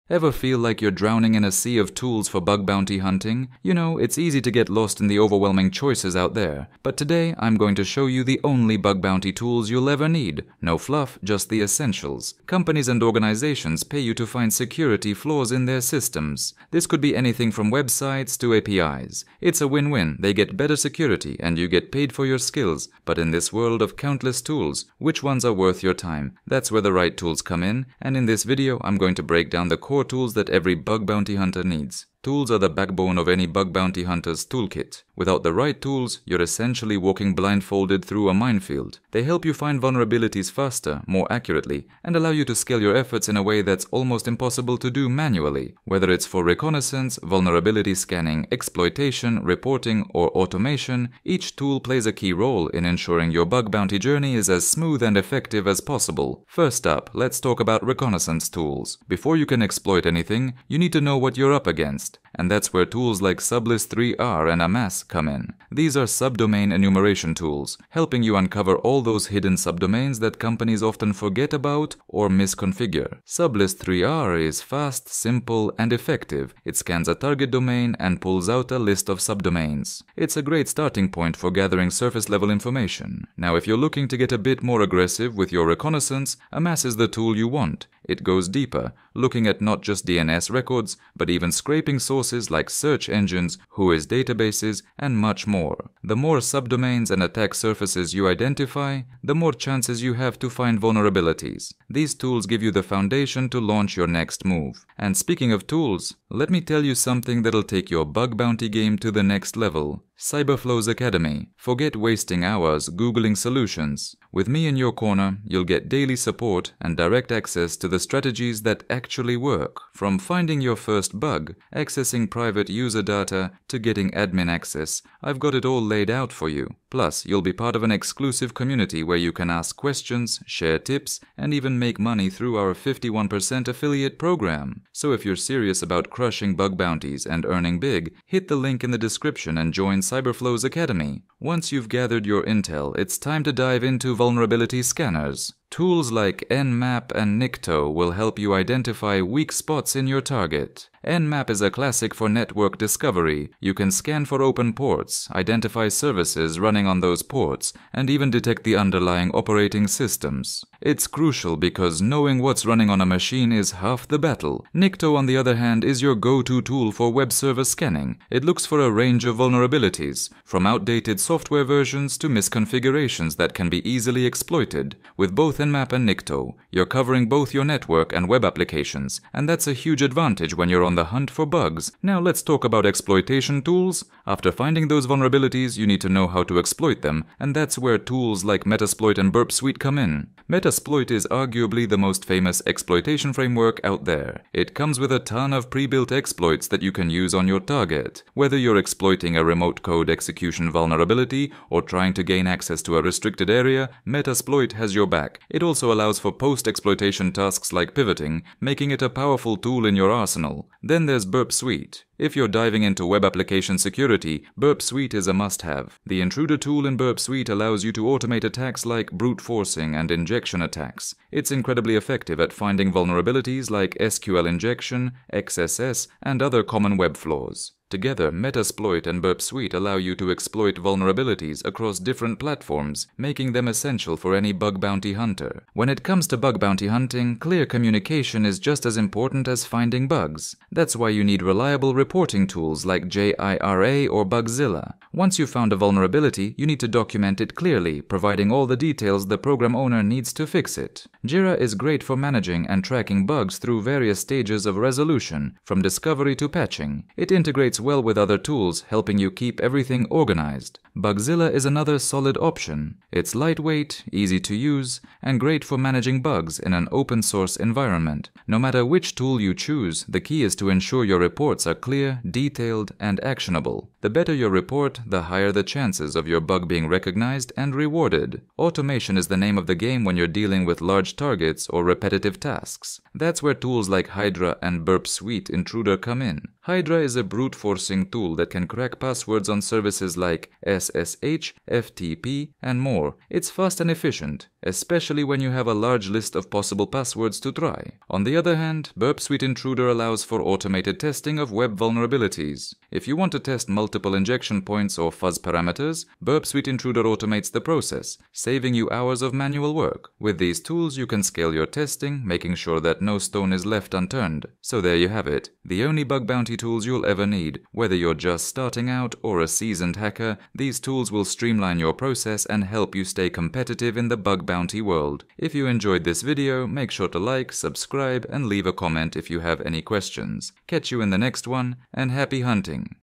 The No-BS Bug Bounty Tools Guide (Everything You Need!)
Join up and get everything you *actually* need to start hacking like a pro 🎓💻✨

Feeling overwhelmed by the endless list of bug bounty tools? This video is your ultimate guide to the essential bug bounty tools every hacker needs! From reconnaissance and scanning to exploitation and reporting, I’ll show you the most efficient tools to find, exploit, and report vulnerabilities. Plus, I’ll share how Cyberflow’s Academy can elevate your bug bounty game with exclusive strategies and direct access to me. Say goodbye to confusion and hello to bug bounty success!

Timestamps:
0:00 - Introduction
0:04 - Why Bug Bounty Tools Matter
0:32 - The Role of Tools in Bug Bounty Hunting
1:00 - Reconnaissance Tools: Sublist3r and Amass
1:45 - Cyberflow’s Academy: Level Up Your Skills
2:21 - Vulnerability Scanners: Nmap and Nikto
3:02 - Exploitation Tools: Metasploit and Burp Suite
3:56 - Reporting Tools: JIRA and Bugzilla
4:38 - Automation Tools: Hydra and Burp Suite Intruder